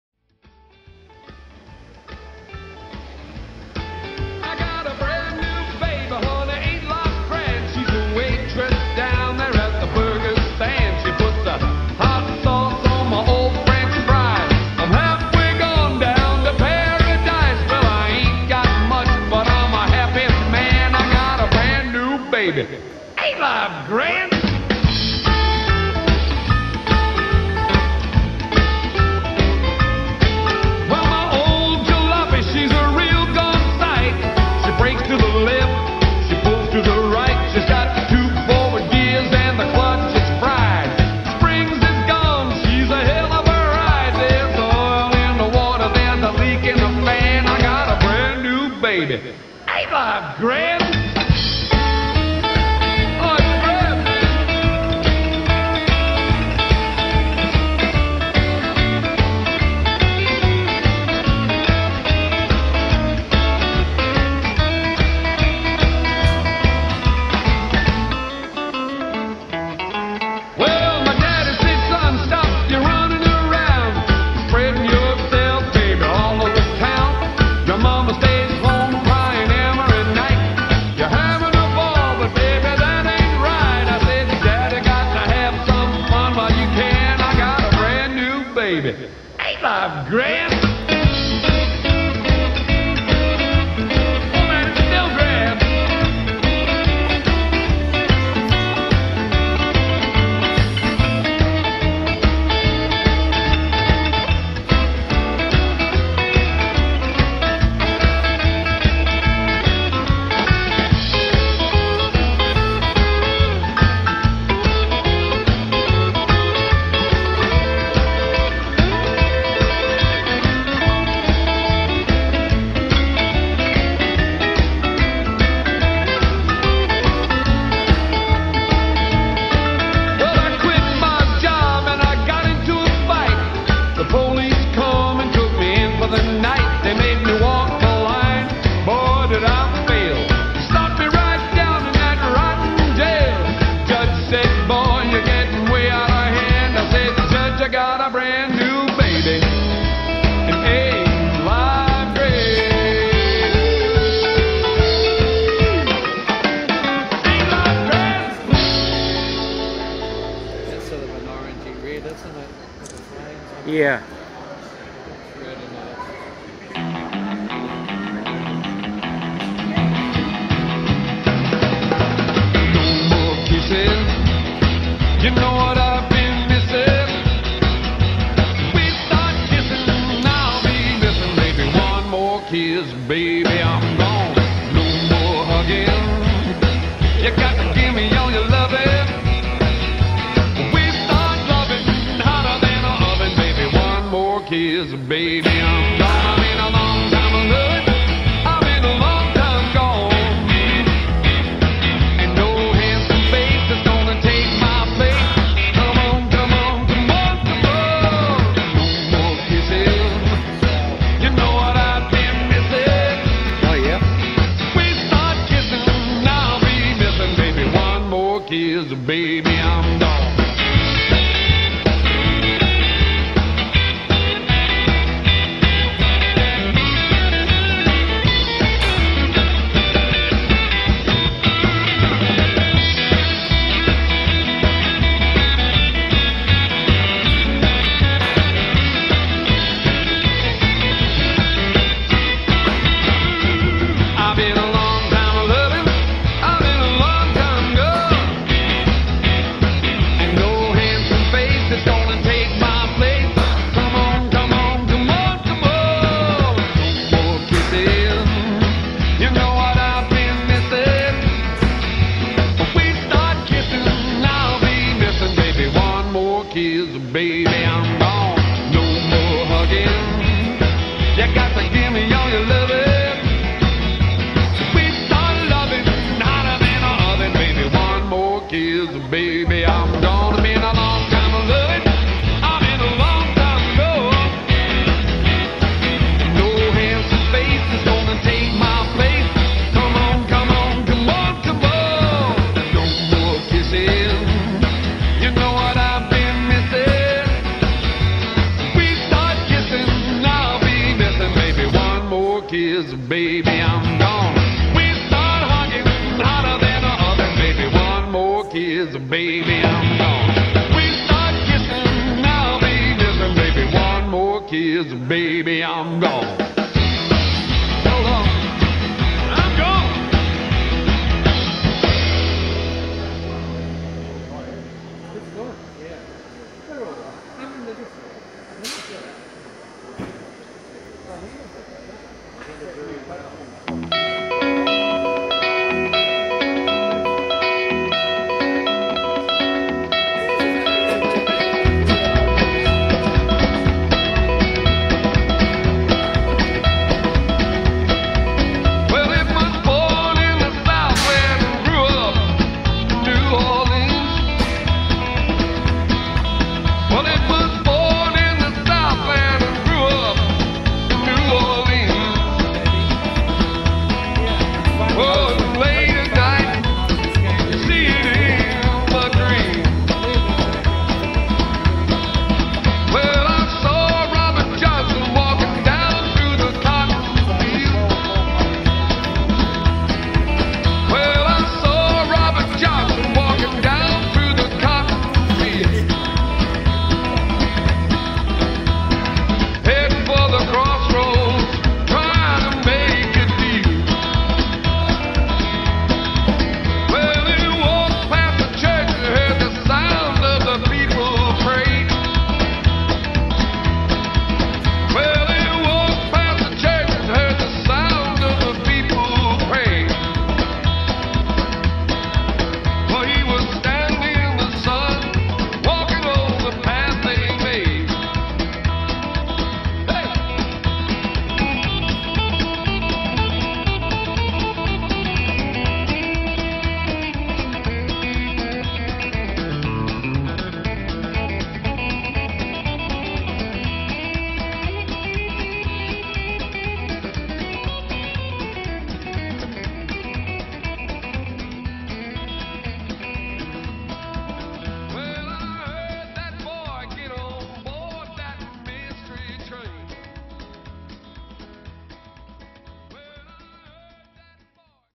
NZ Hot Rod Show 2014 Part 2.
Held at The Edgar Centre Dunedin over Labour Weekend 2014 More Favourites videoed on my Lg G3 SmartPhone. For a HD Video click on the wheel under the pic above .Quality ...then 1080p This Video was recorded in 4K then downsized to 1080p.